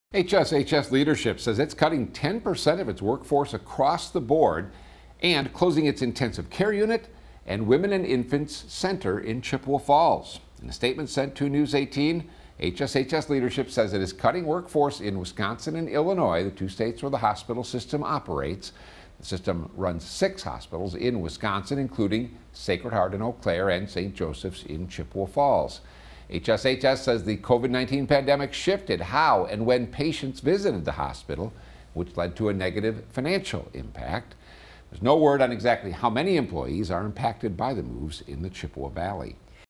HSHS system, including Sacred Heart and St. Joseph’s cuts 10% of staff, some local departments close
Hospital Sisters Health System (HSHS), with hospitals in Eau Claire and Chippewa Falls, is cutting 10% of its workforce and closing its intensive care unit and Women and Infants Center in Chippewa Falls.